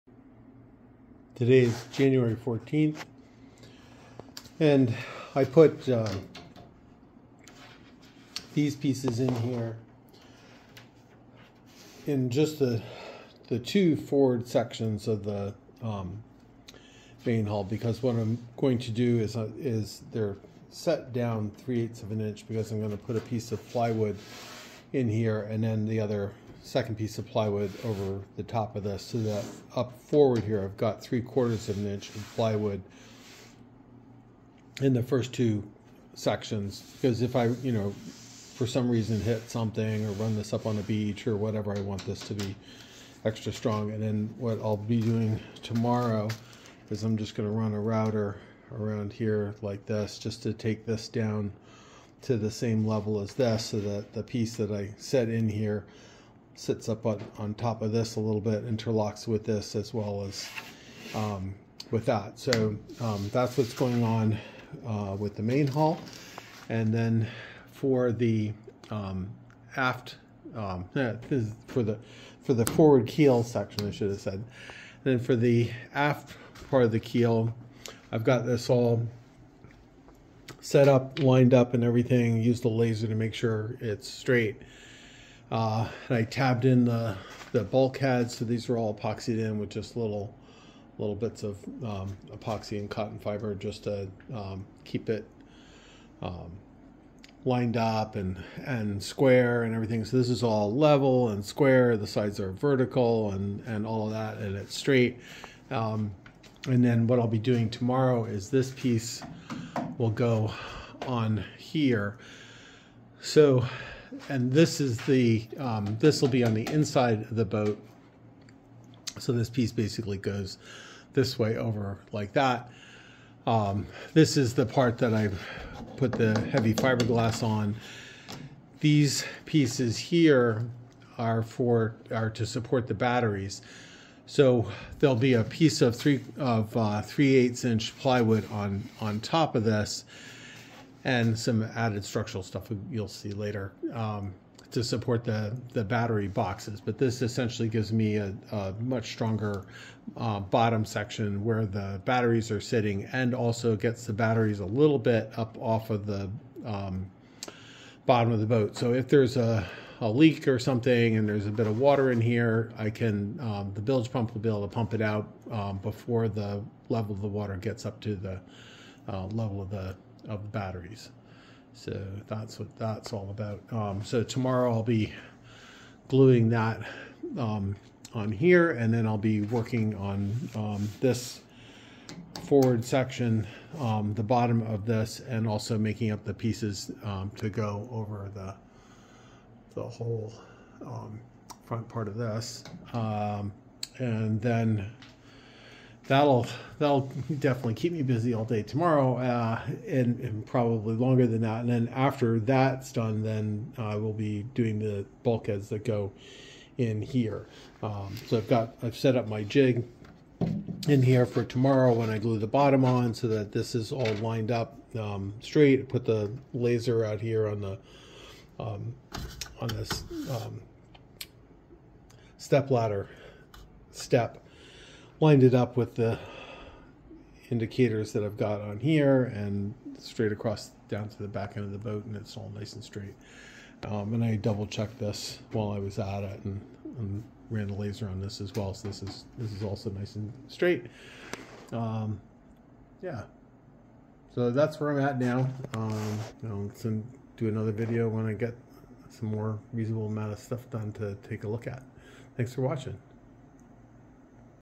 Electric Boat Construction video 13
Forward keel section has been flipped over. I am starting to work on the bottom.
Aft keel section is ready to have the bottom put on. Some of the battery support pieces are in place.